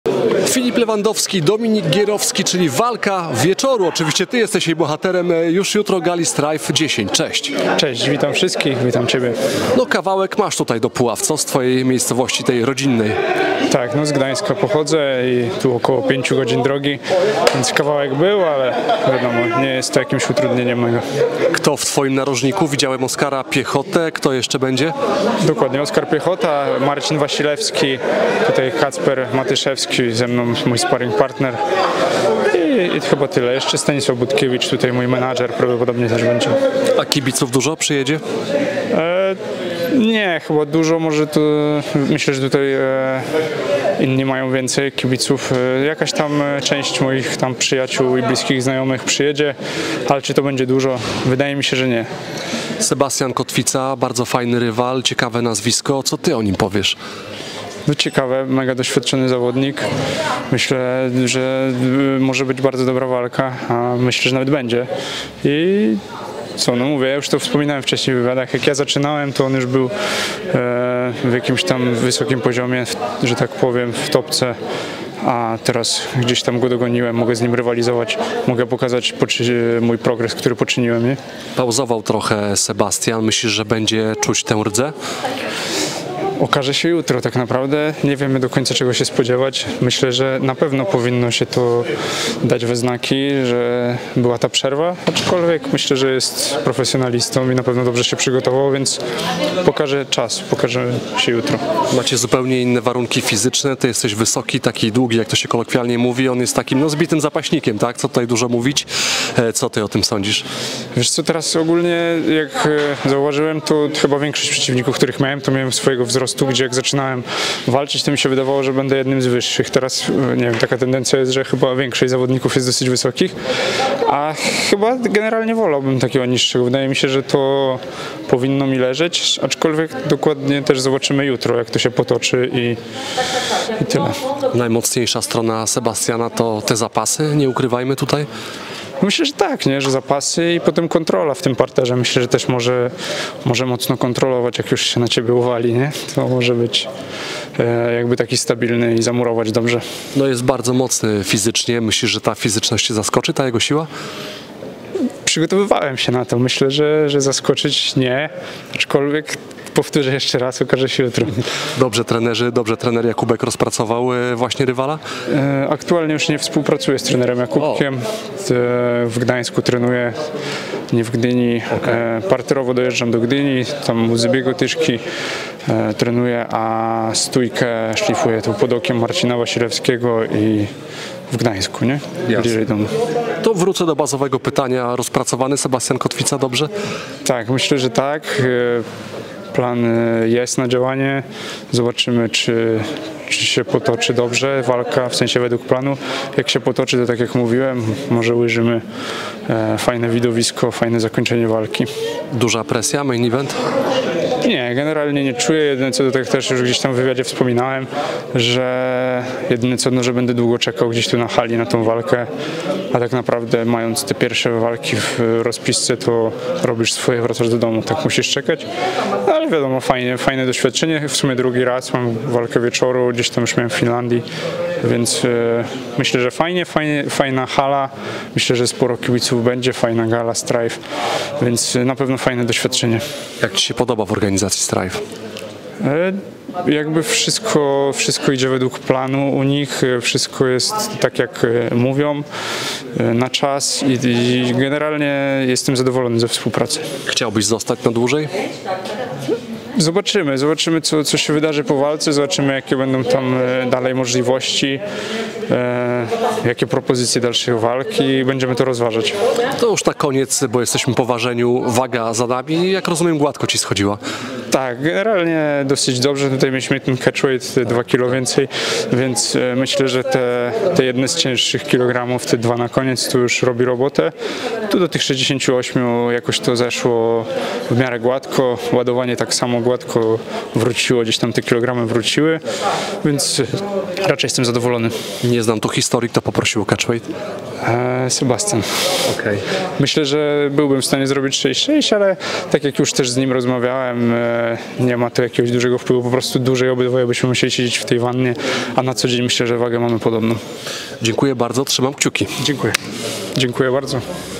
To Sebastian Kotwica chciał catchweight - Dominik Gierowski przed STRIFE 10
Wywiady po oficjalnym ważeniu przed galą STRIFE 10. Bądźcie jutro z nami!